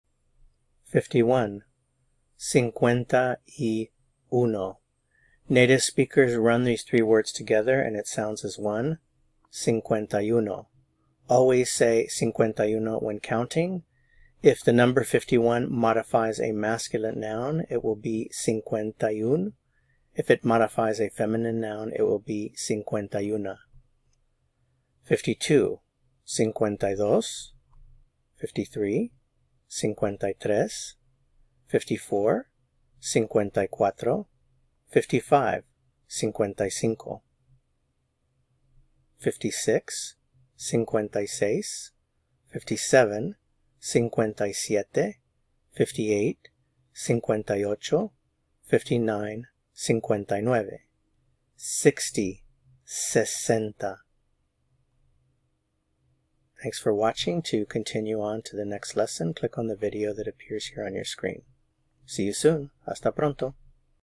0071 Easily Learn Spanish, NUMBERS 51 - 60
Basic numbers in Spanish. Lesson 71
Learn the numbers from 51 to 60 in Spanish with careful pronunciation.